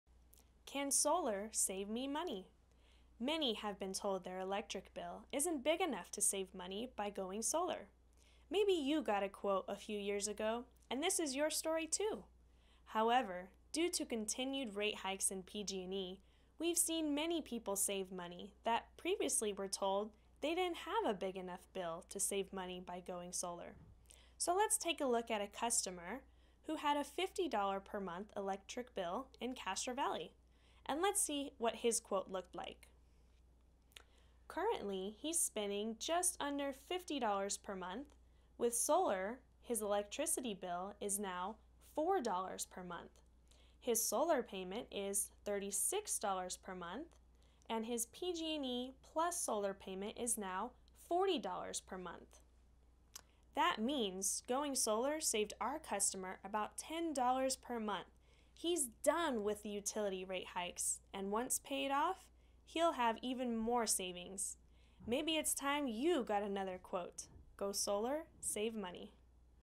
Can Solar Save Me Money?
We are solar energy professionals. Can solar save me money even if my bill isn't huge? Compare your numbers with one of our Castro Valley customers.